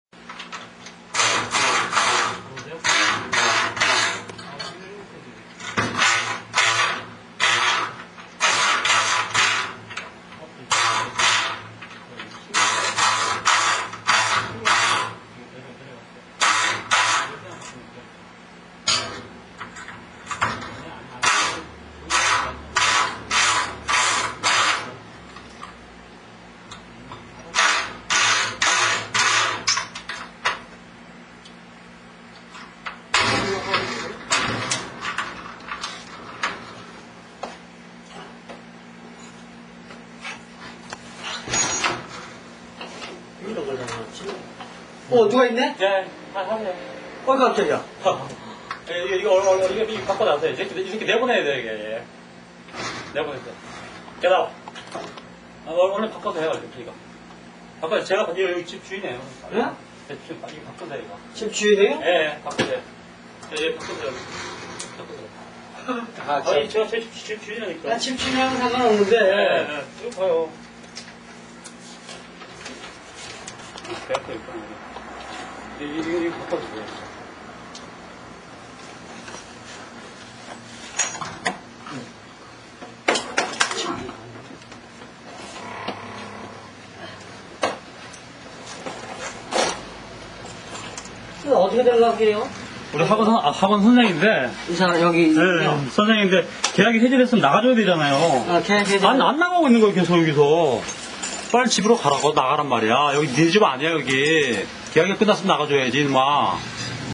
Illegal Break-in Home Break-in via Locksmith Part1
My Korean Hagwon(English School) Owner fired me illegally, didn't pay me, broke into my home, etc...Notice how the locksmith is surprised someone is inside but he doesn't really care since he's getting his money and the BAD and typical KOREAN HAGWON OWNER lies to him.